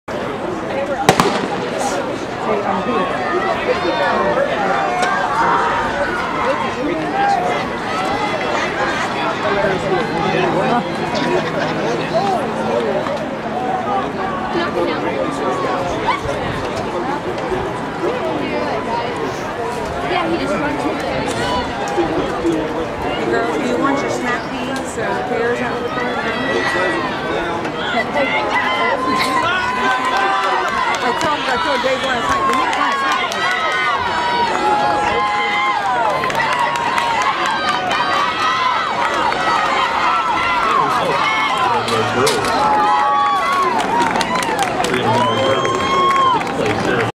300IH 1ab Heat 1
2018 West Virginia State Track Meet - Class A Boys 300 Meter Intermediate Hurdles - Heat 1